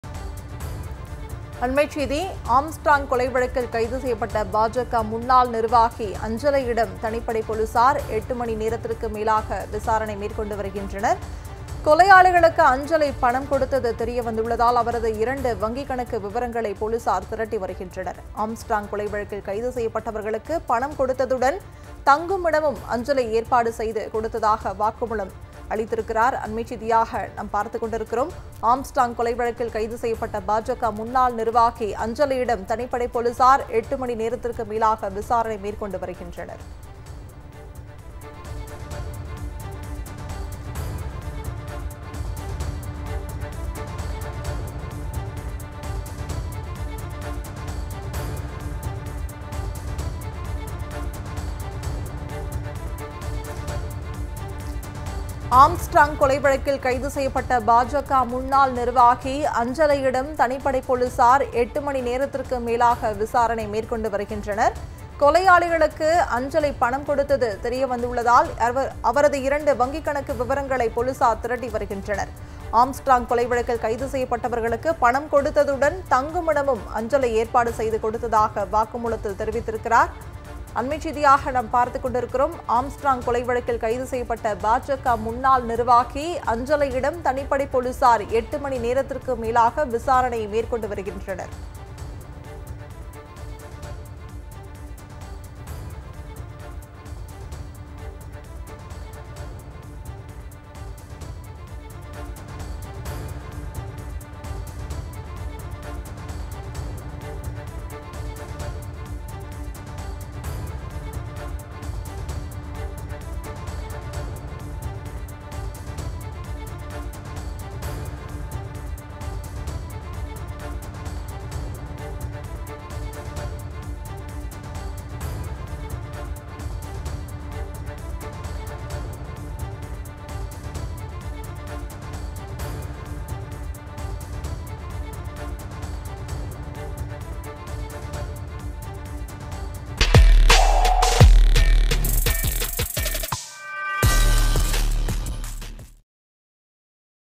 BREAKING | ஆம்ஸ்ட்ராங் கொலை வழக்கு : பாஜக முன்னாள் நிர்வாகியிடம் தீவிர விசாரணை | TN Police
ஆம்ஸ்ட்ராங் கொலை வழக்கு : பாஜக முன்னாள் நிர்வாகியிடம் தீவிர விசாரணை  | Kalaignar News